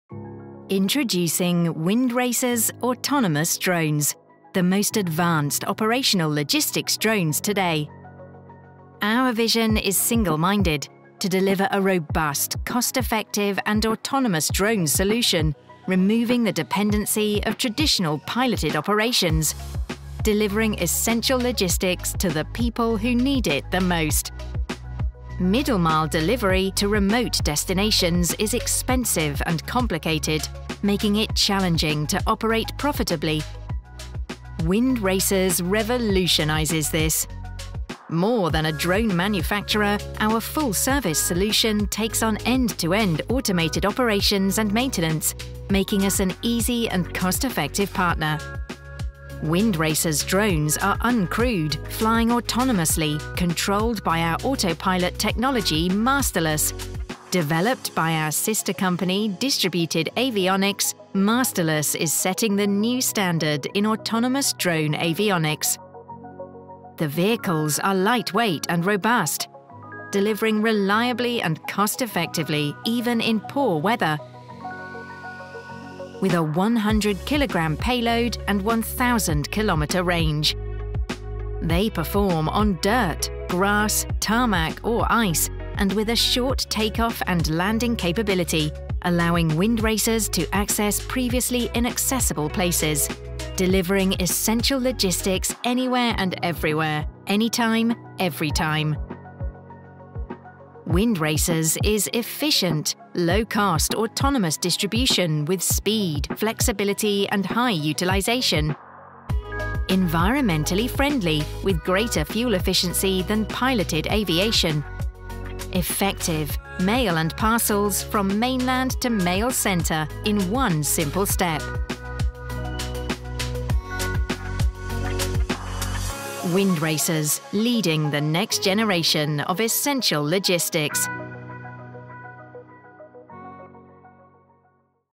Windracers Autonomous Drones Delivering Essential Logistics
Windracers autonomous drones are the most advanced operational logistics drones today. With a payload of 100 kg and a range of 1,000 km, we are able to deliver essential logistics anywhere and everywhere, any time, every time.